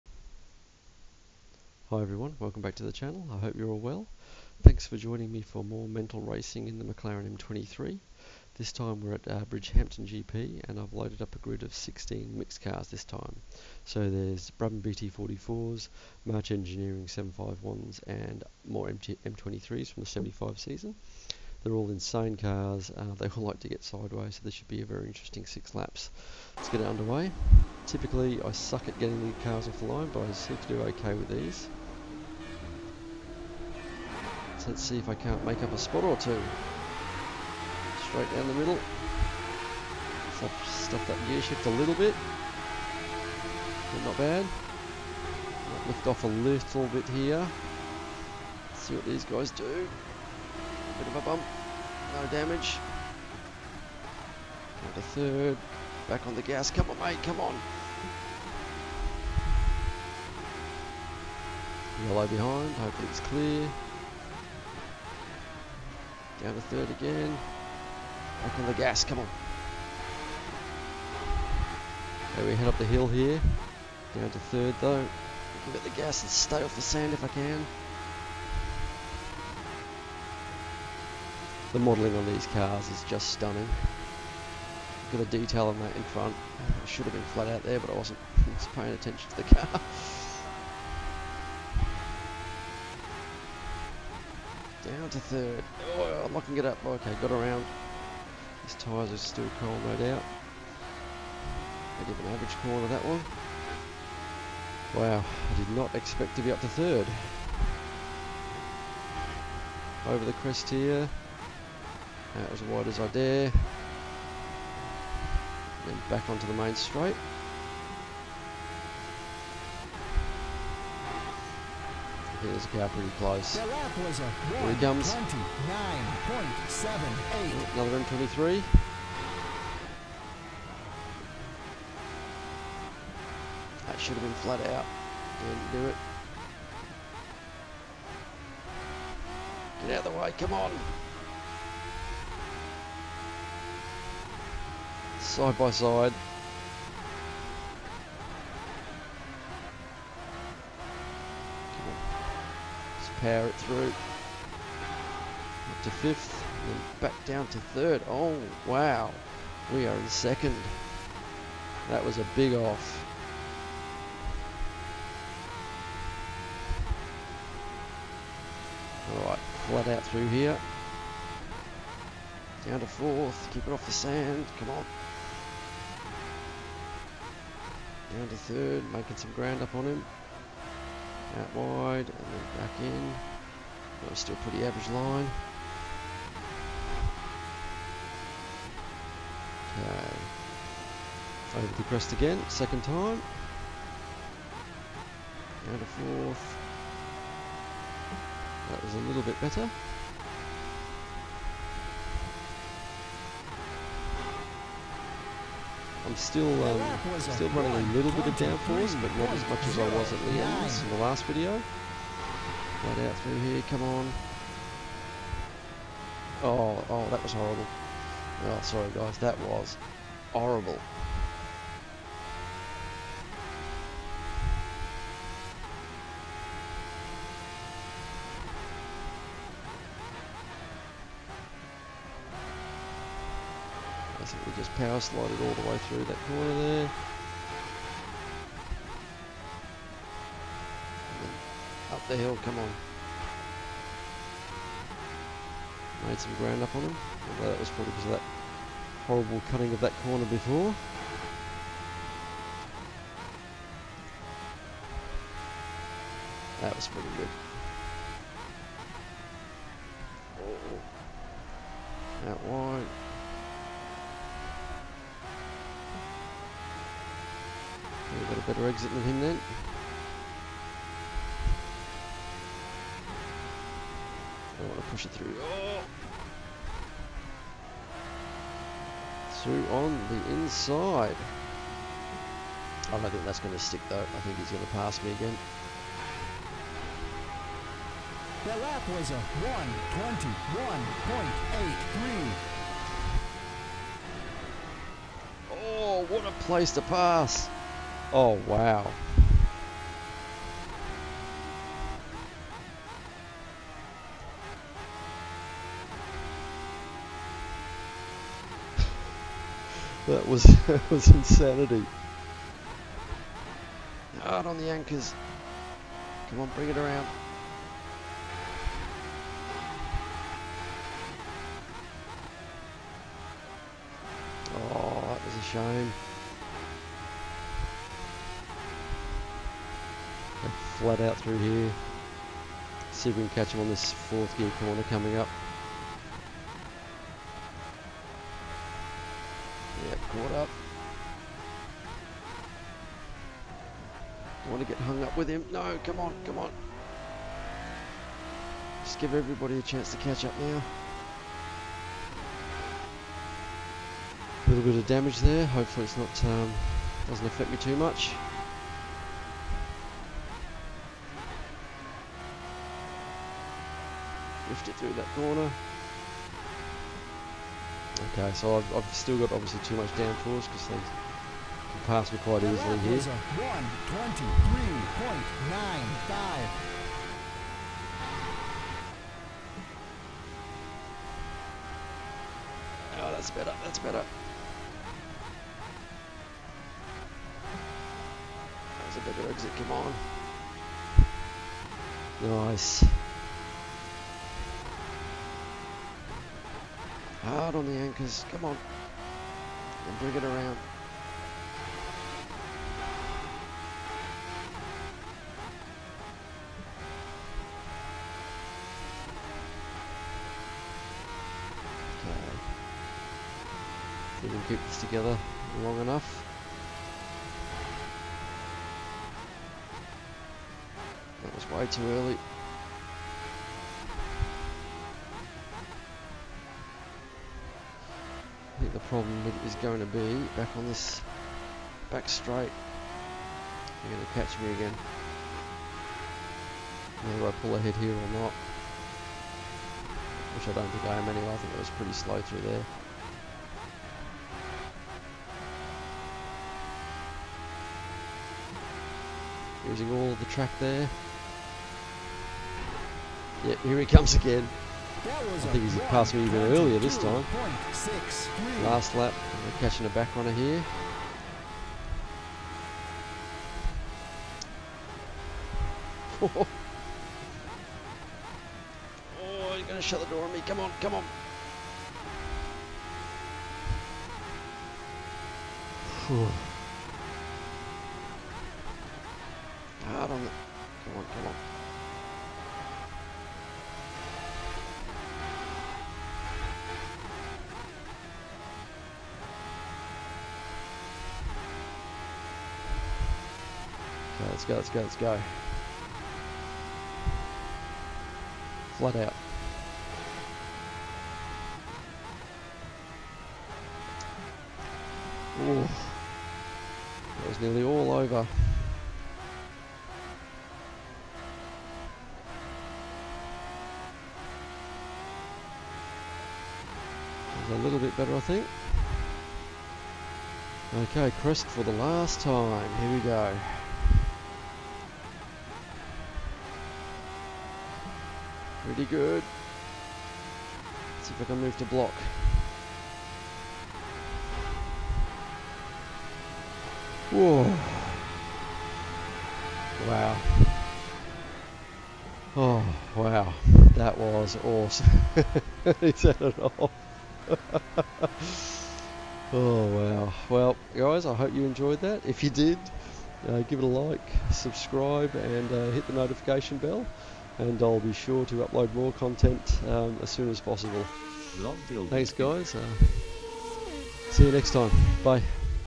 MORE MENTAL!
More racing in the McLaren M23, this time at Bridgehampton against Brabham and March Engineering 75 cars. Insane racing, hope you all enjoy.